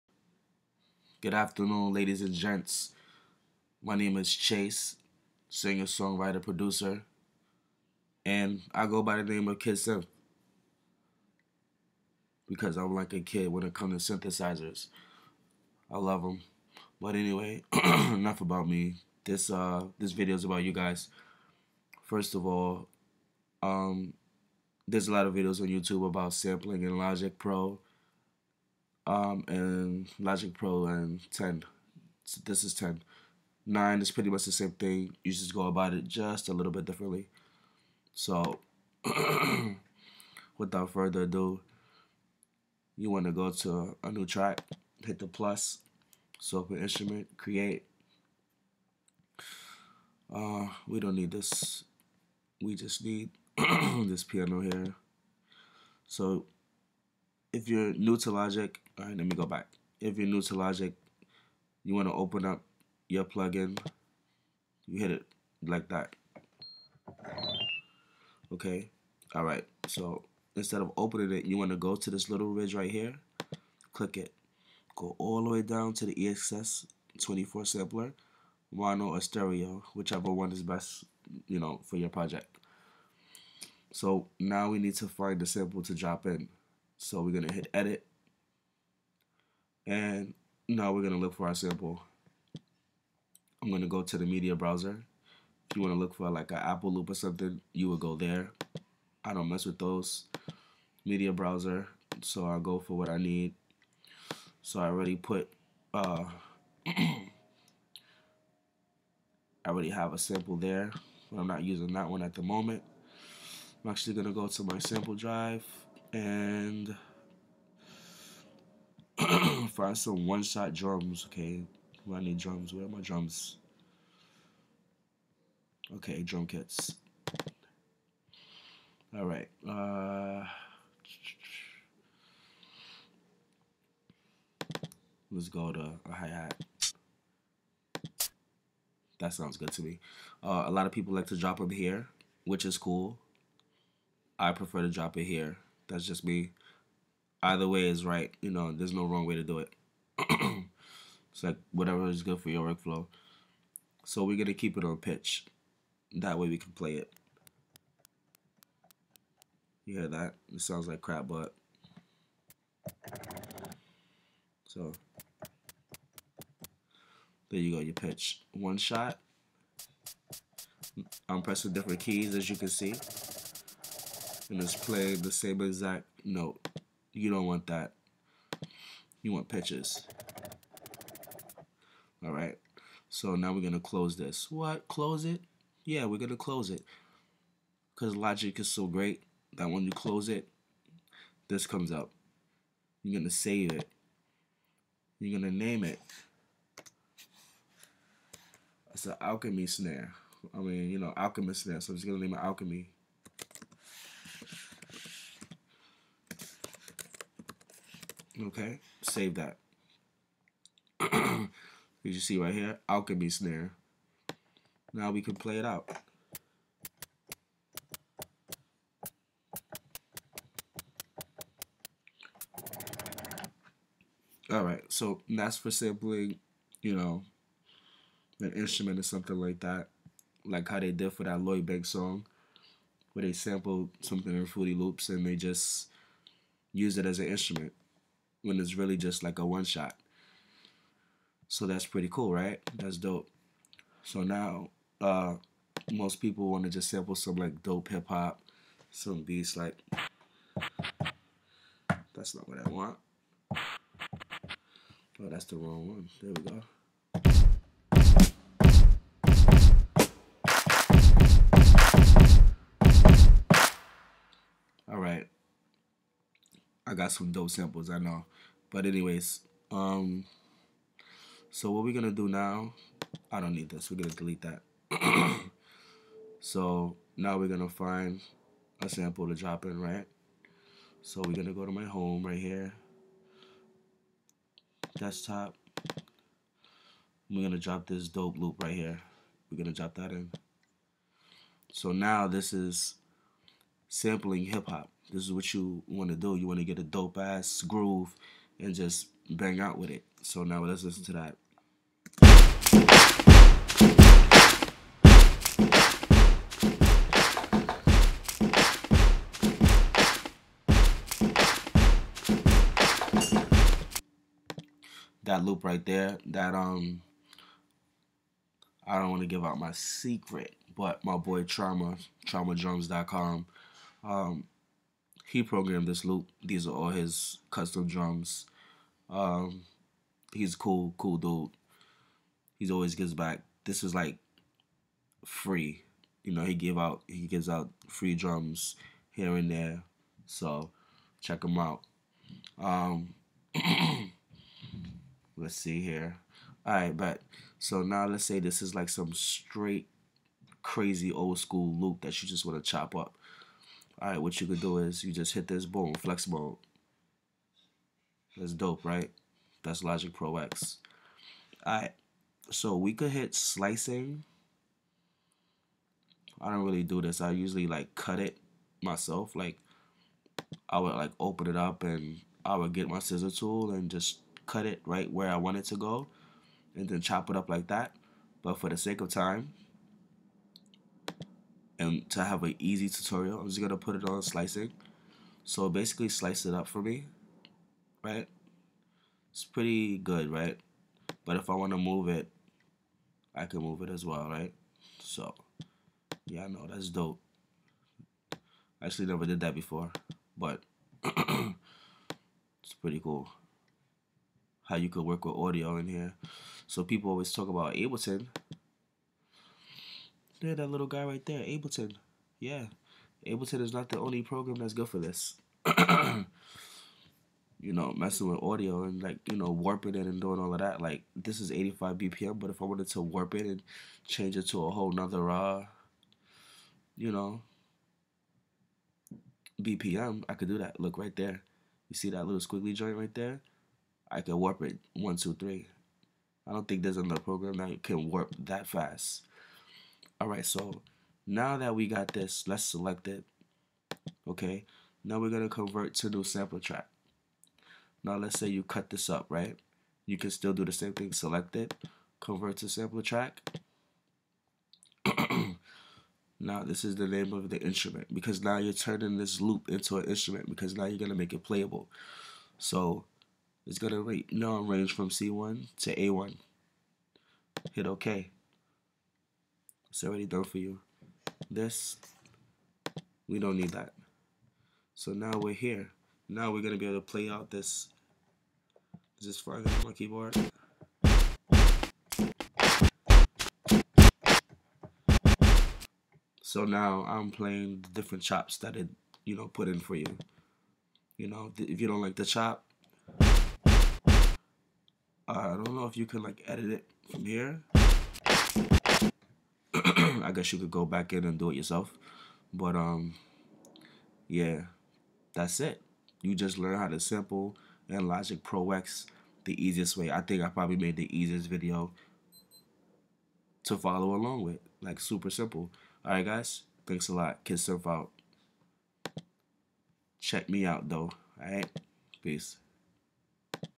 Logic Pro X EXS24 Sampling Tutorial Simplified
JohnnySav shows us how to sample instruments and dope grooves in Logic Pro X.